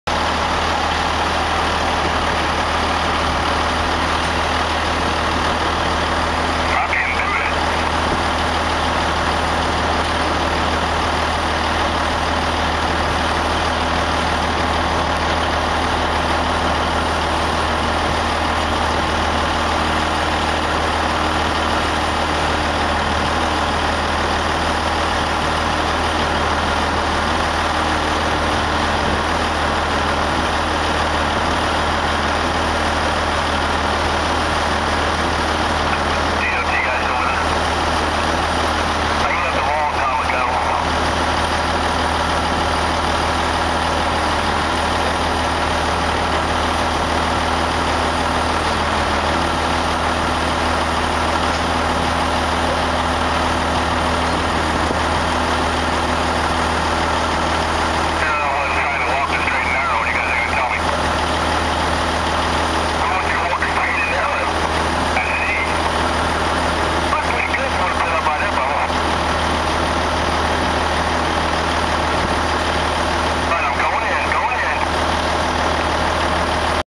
Beyel Brothers Heavy Haul
24 axle  450 lbs 22' wide 264' long Nebarska to Houston TX